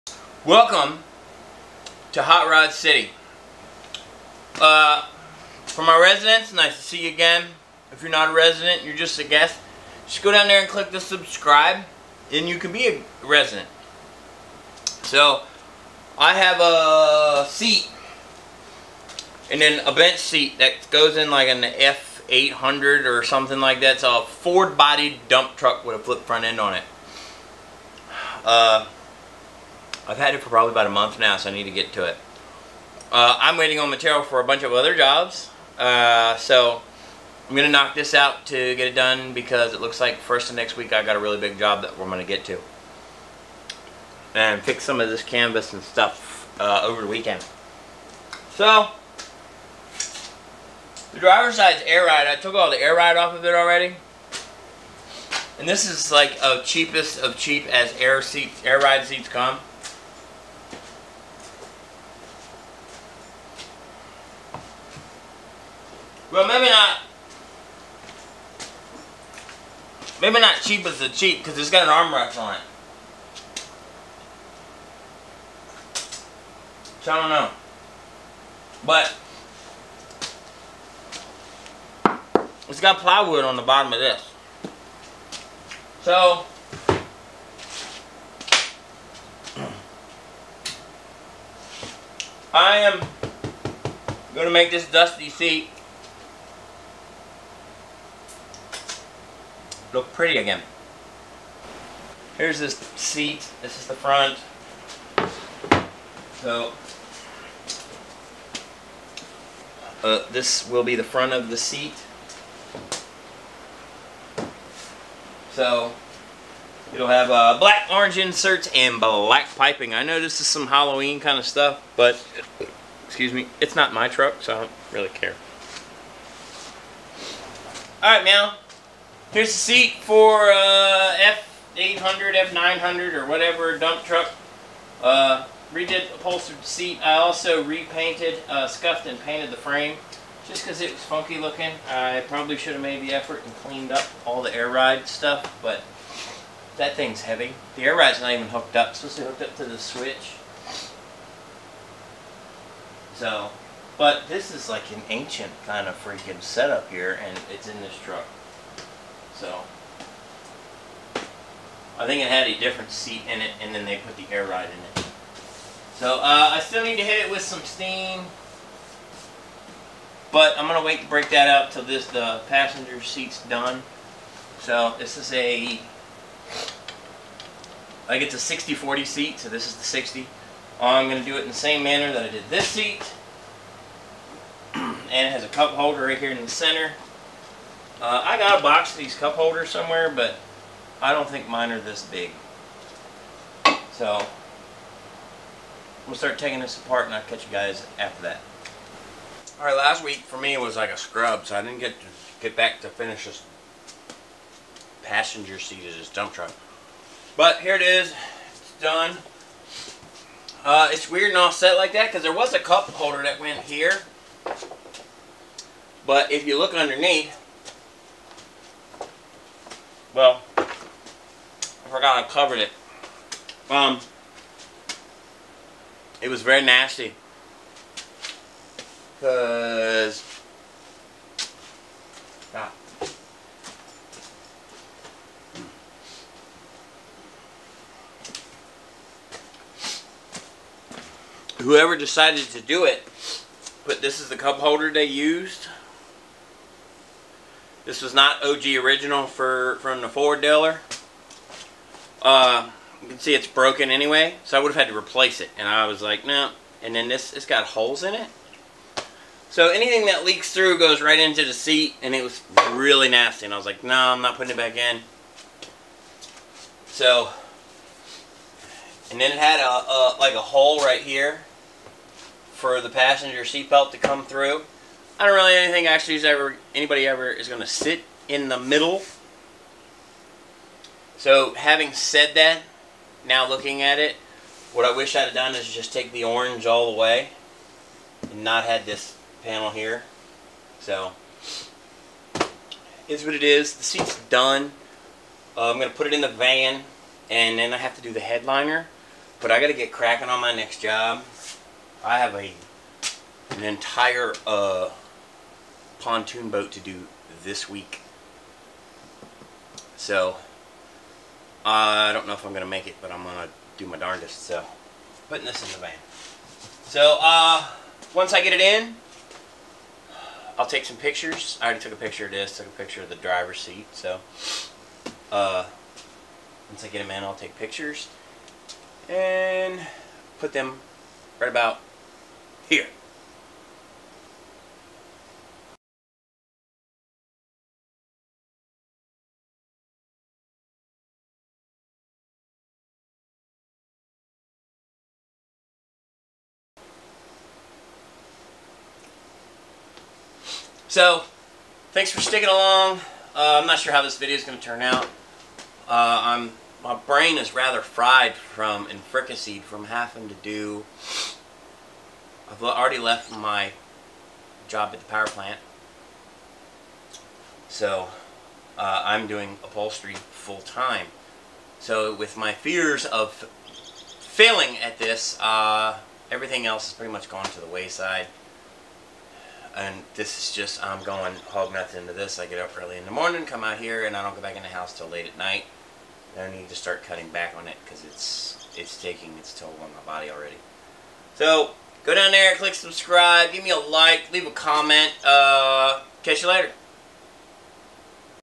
F800 Dump Truck Seats
In this episode of Hot Rod City, I reupholster some seats out of a Ford f800 single axle dump truck